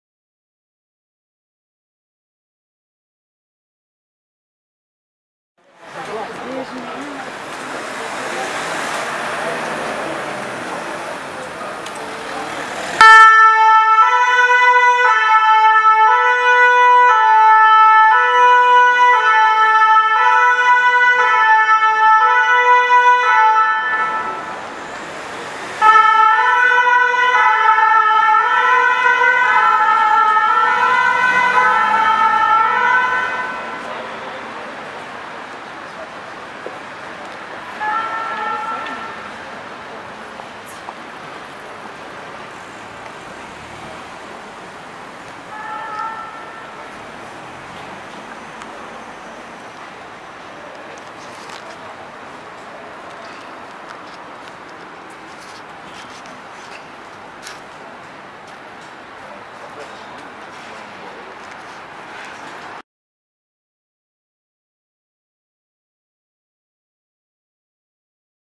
VSAV CS ROUSSEAU 8EME CIE BSPP
VSAV ROUSSEAU 8 EME COMPAGNIE DÉCALE EN URGENCE DANS LE
1ER ARRONDISSEMENT DE PARIS .